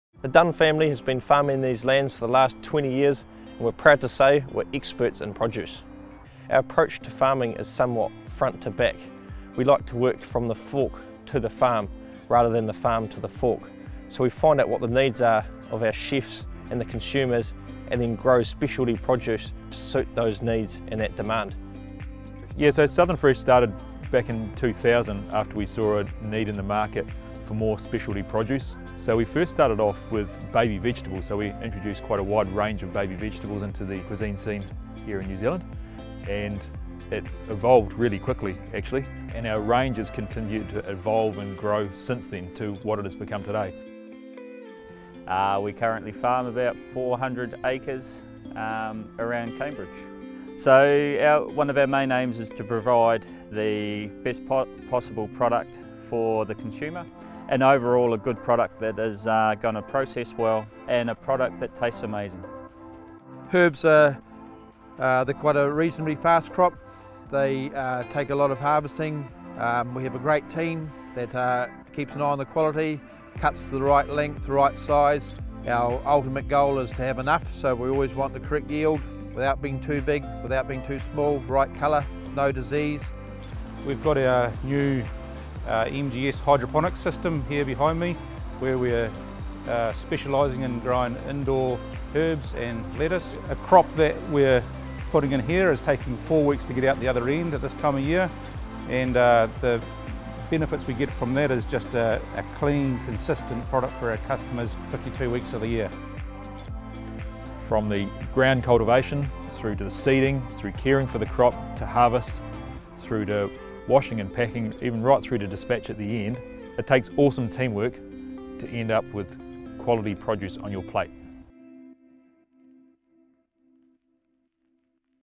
Company Overview | Southern Fresh Foods
We travelled up to the mightly Waikato to catch up with Brock and the team from Southern Fresh Foods.